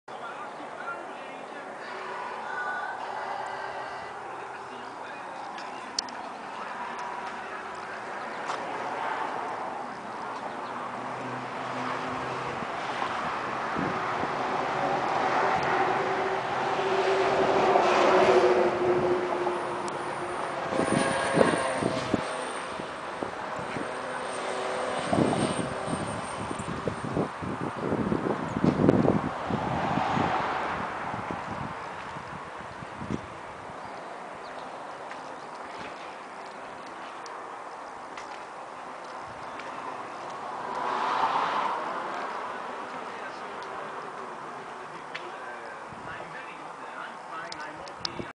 Achat | Vente Caravane de voyage | Bullet 308 BHS l 2015, stock KB-058, vue de l'extérieur
Vidéo vue de l'extérieur d'une Caravane de voyage | Bullet 308 BHS l 2015, stock KB-058, disponible chez Roulottes Lupien